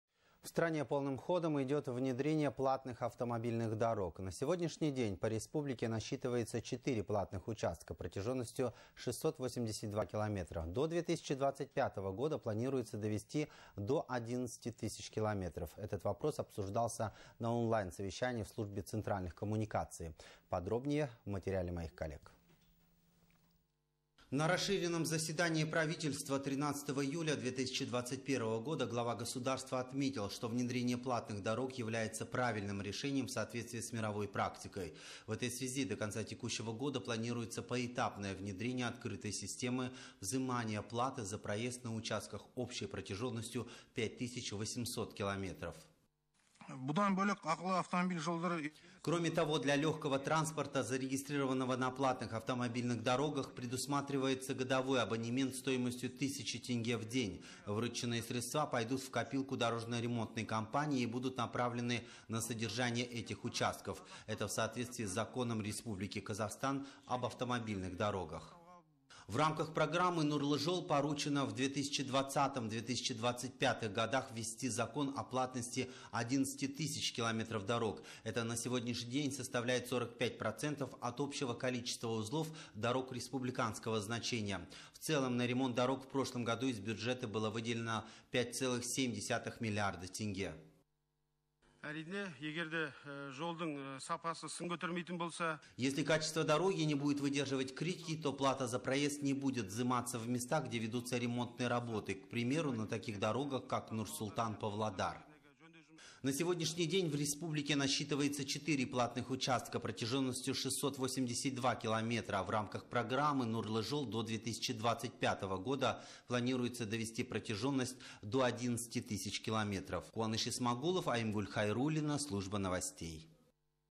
В стране планируется довести платные автомобильные дороги до 11 000 км
В стране полным ходом идет внедрение платных автомобильных дорог. На сегодняшний день по республике насчитывается 4 платных участка, протяженностью 682 км. До 2025 года планируется довести до 11 000 км. Этот вопрос обсуждался на онлайн совещании в Службе центральных коммуникаций. подробнее в материале моей коллеги.